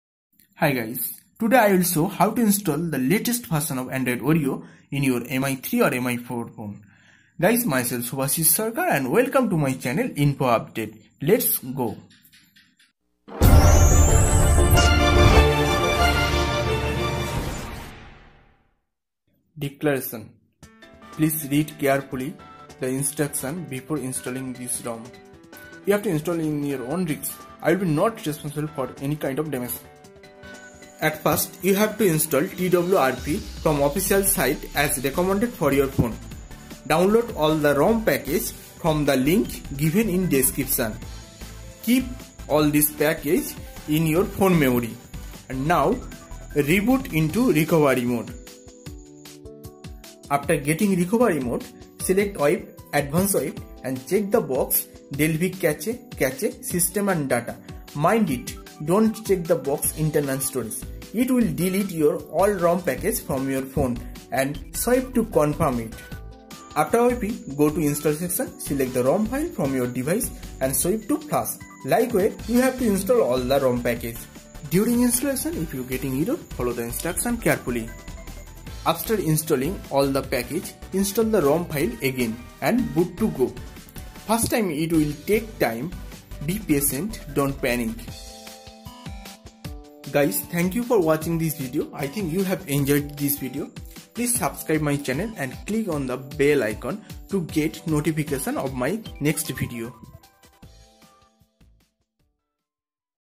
Install Android 8.0 Oreo on Xiaomi Mi4/Mi3
hey guys in this video i'll show how to install android oreo 8.0.0 in xiomi mi4 and oreo 8.0.0 in xiomi mi 3 [cancro] !! oreo mi 4 , oreo mi 3
1) You can have any TWRP installed, I have the oficial one.
2) Wipe data, cache, etc in the recovery.
3) Important step: Reboot to recovery after wiping data, cache etc.
4) After the recovery opens again, just flash the ROMzip directly.
5) Flash magisk zip
6) Flash gapps zip. This will take some time. Just be patient and let the recovery reboot itself.
7) After recovery opens again, just reboot to system.
8) Initial boot takes some time.
9) Just skip any initial setup as keyboard crashes and google account can't be added right away.
10) You will be welcomed by the home screen.
11) Install a new keyboard app.
12) Go to settings and add Google account from there.
Hope it helped. I have tried to keep it simple and easily understandable for anyone who is no very fluent in all the flashing and stuff.
ROM OS Version: 7.x Nougat
ROM Kernel: Linux 3.10.x
Based On: Pure Aosp
Whats working?
Phone boots luckily
Ril
Camera
Wifi
Bluetooth
Sensors
Everything that is improtant
Whats not?
Nothing major
Extra stuff added
Browser
Gboard
music
Download Link